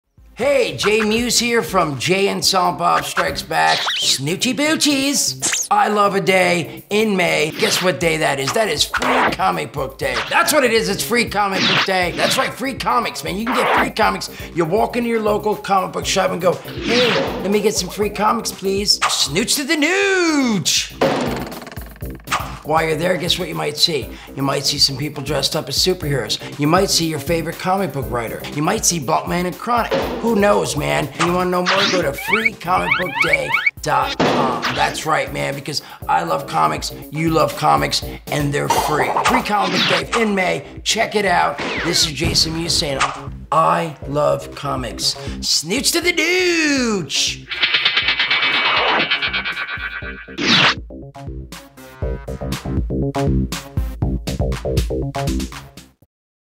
Jason Mewes Promotes Free Comic Book Day for Diamond Comic Distributors
Promo produced, directed, and edited by Gas Light Pictures